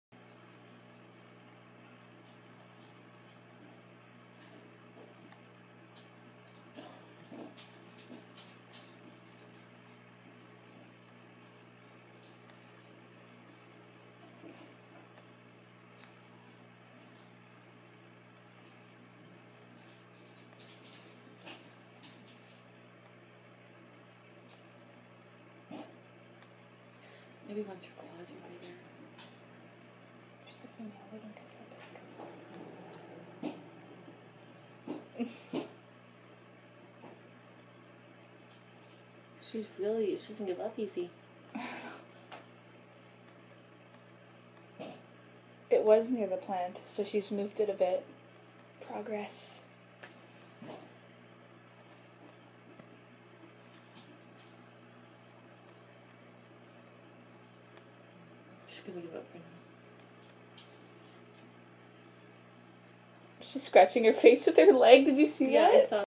Crayfish trying to pick up her toy
This is my baby electric blue crayfish trying to pick up her lady bug toy 😂